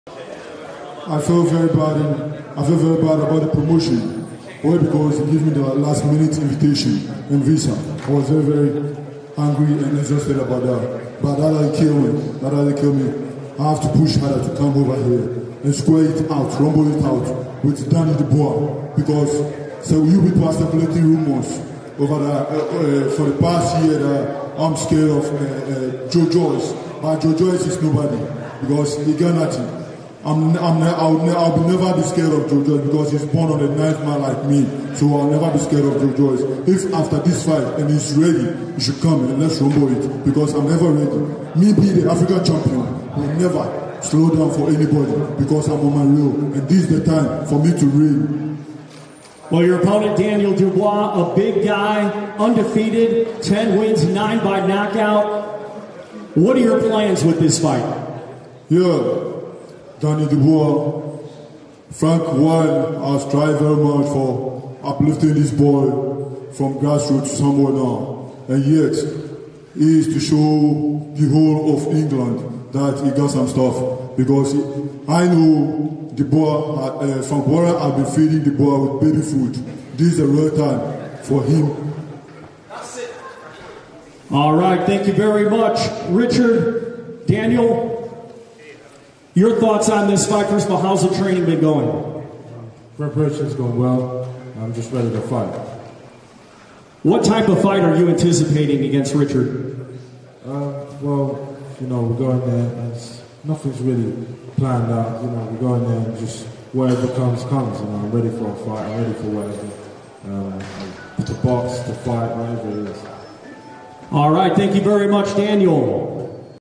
Richard Lartey rant after Dubois face-off | "Frank Warren has been feeding Daniel Dubois baby food!"
Richard Lartey and Daniel Dubois faced off and then exchanged words on stage ahead of their fight on Saturday night.

Lartey also fired shots at Joe Joyce who has just signed a deal to fight on BT Sport...

Read more on upcoming fights on BT Sport and BT Sport Box Office ➡️